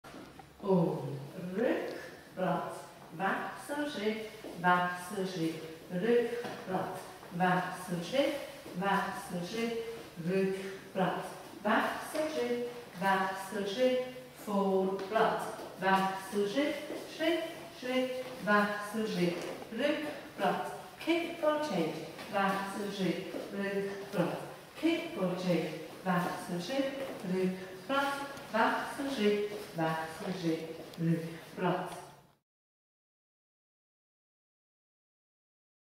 Jive Kombination für Fortgeschrittene - Katapult mit Challange
Das nächste Video unserer Fortgeschrittenen-Reihe Jive Figuren. In dieser Reihe gibt es jeden Monat eine neue Jive Kombination zum Nachtanzen für Fortgeschrittene. Viel Spass dabei!
Falls du Feuer gefangen hast, findest du viele Tanzkurse und Tanzstile Live im Danceorama Bern und Solothurn